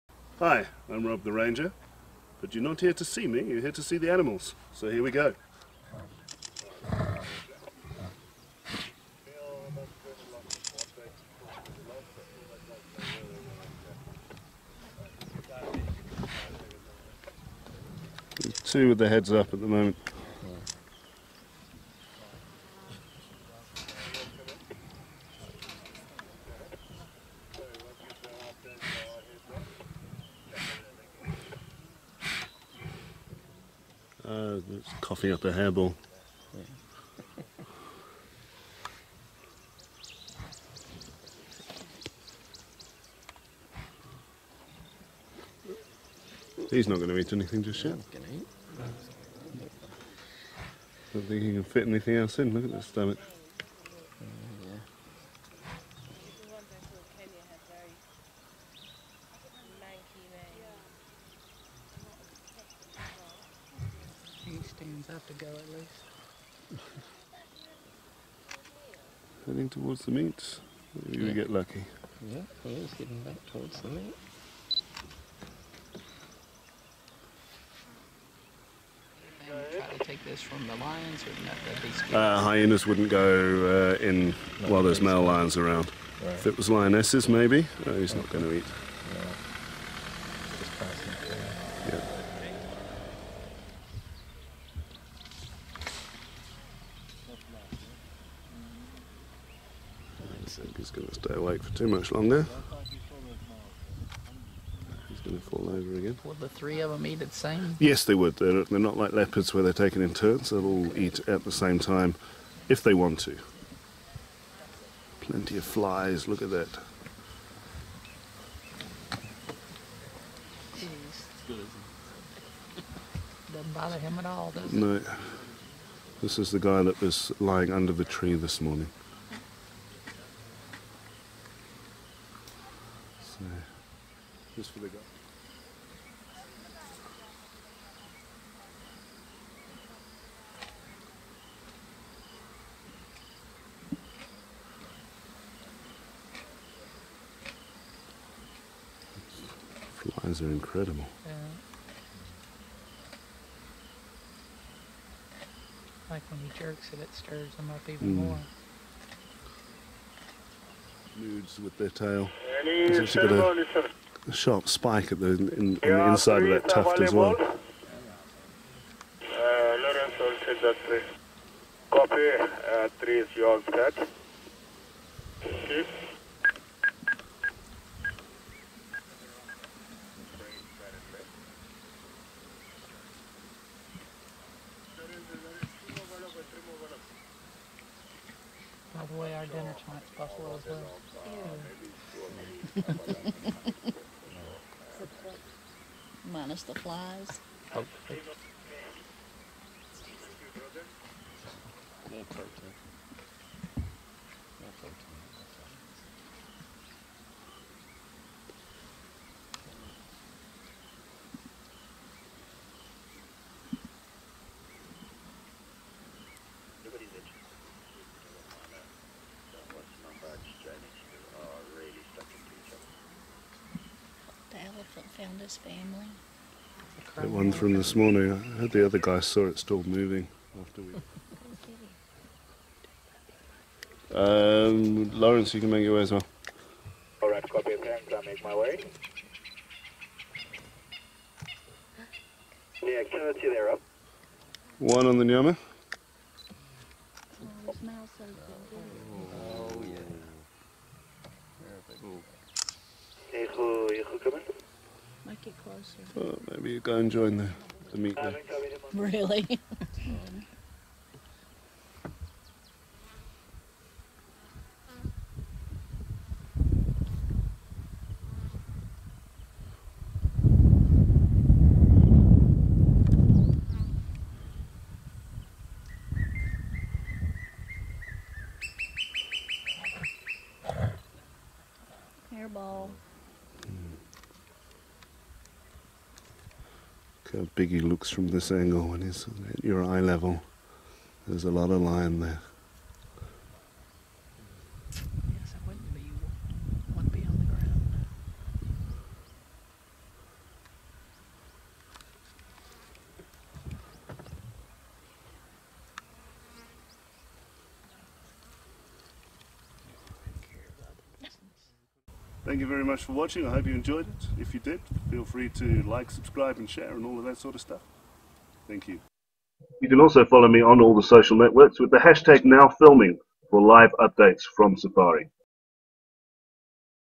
3 Big Lions Eat African Buffalo In South Africa
The Majingilane lion coalition lazing around with a huge buffalo meal.